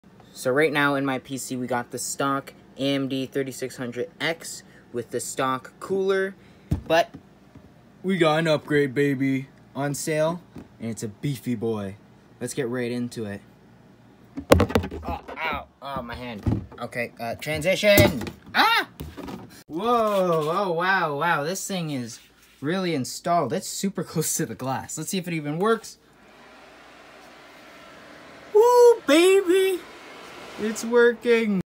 Upgrading My Gaming Computer - MAXIMUM Cooling!!! - Best Air Cooler?
Corsair a500 is a really good cooler
Water cooling is a little too pricey. so I went with some good old fashioned air cooling

Topics: a500 corsair air cooler,best air cooler,building a computer,pc gaming,gaming pc,intel,ryzen,water cooling is trash,corsair a500,corsair,cpu cooler